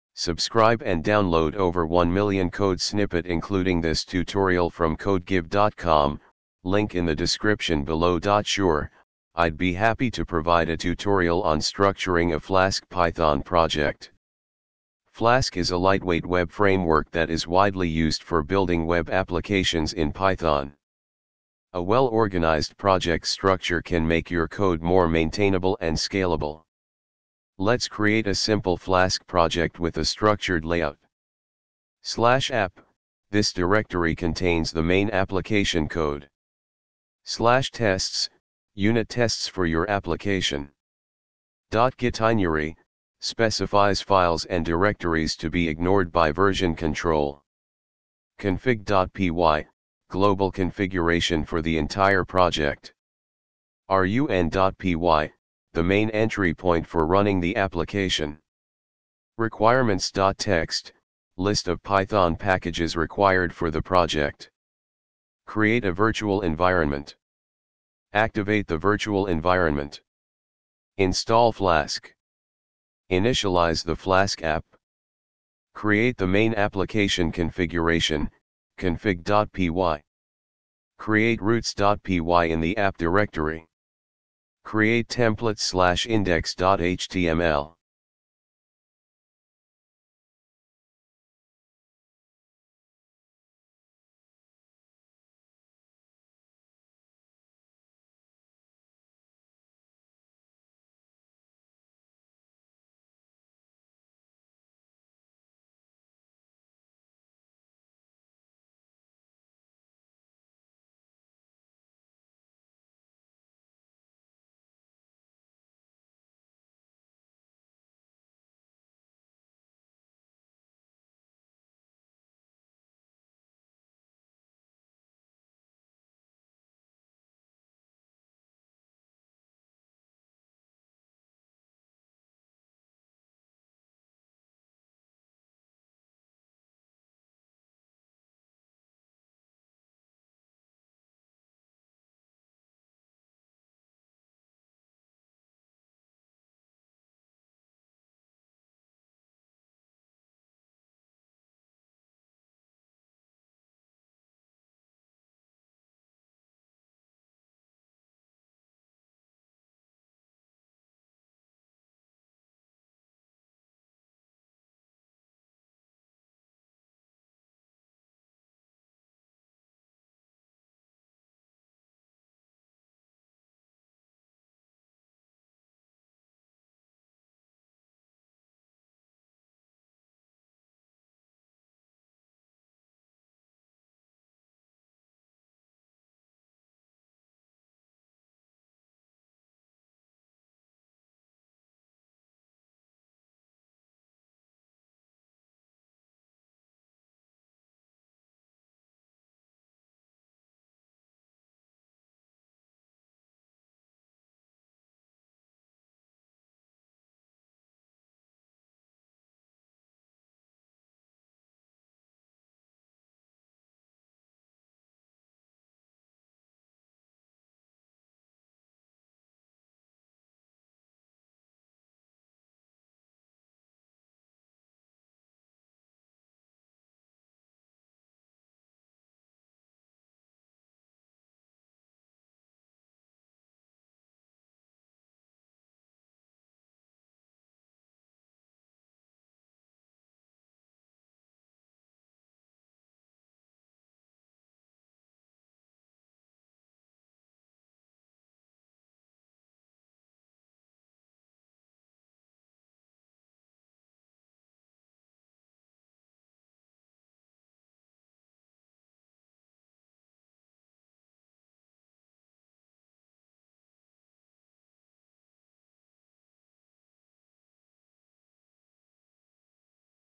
flask python project structure
Sure, I'd be happy to provide a tutorial on structuring a Flask Python project. Flask is a lightweight web framework that is widely used for building web applications in Python. A well-organized project structure can make your code more maintainable and scalable. Let's create a simple Flask project with a structured layout.
/app: This directory contains the main application code.
/tests: Unit tests for your application.
.gitignore: Specifies files and directories to be ignored by version control.
config.py: Global configuration for the entire project.
run.py: The main entry point for running the application.
requirements.txt: List of Python packages required for the project.
Create a Virtual Environment:
Activate the Virtual Environment:
Install Flask:
Initialize the Flask App:
Create the Main Application Configuration (config.py):
Create routes.py in the app directory:
Create templates/index.html: